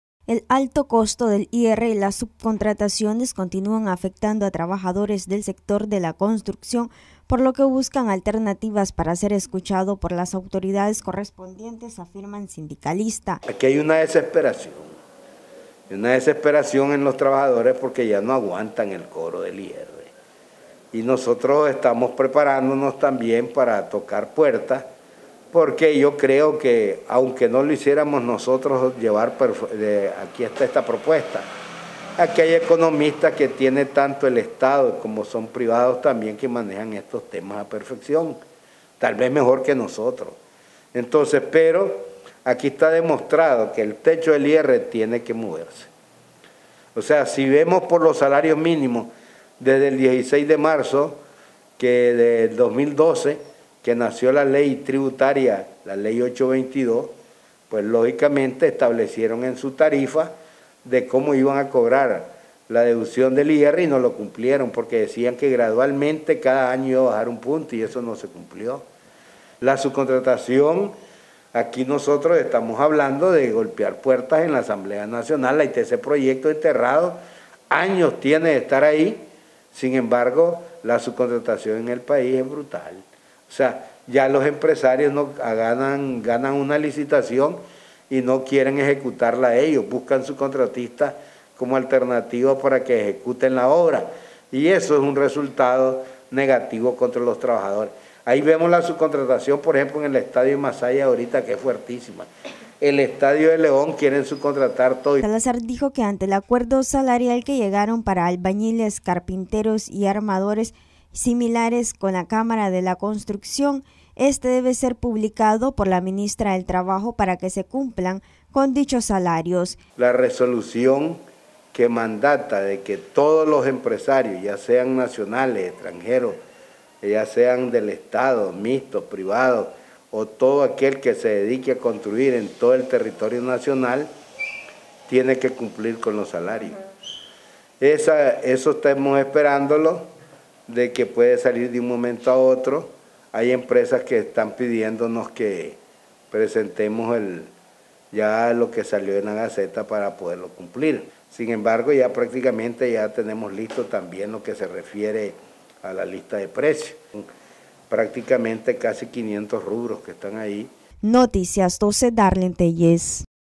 IR y subcontrataciones afectan a trabajadores de la construcción.
Noticias12Nicaragua Sindicalistas solicitan a las autoridades correspondientes, que se revise el techo del IR y que se dé una solución a las subcontrataciones que aquejan al sector.